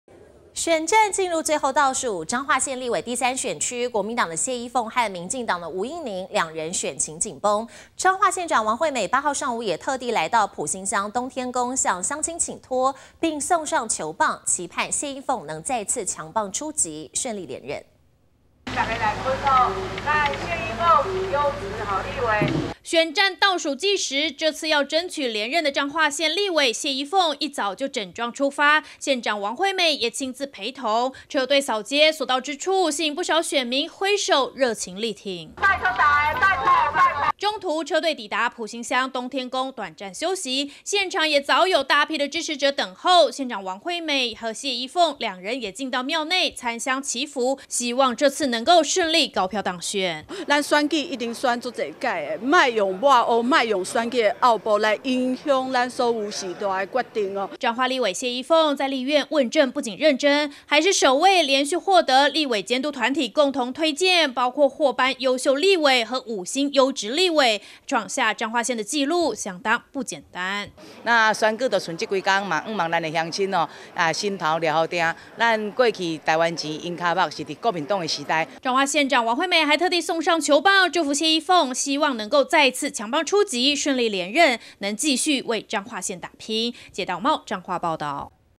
選戰衝刺！ 彰縣長王惠美陪謝衣鳯車隊掃街
選舉進入最後倒數一周！彰化縣立委第三選區，國民黨的謝衣鳯，和民進黨的吳音寧，兩人選情緊繃，彰化縣長王惠美，8號上午，也特地來到埔心鄉東天宮，向鄉親請託，並送上球棒，期盼謝衣鳯能再次強棒出擊，順利連任，而2人車隊掃街，所到之處，也受到不少選民熱情支持！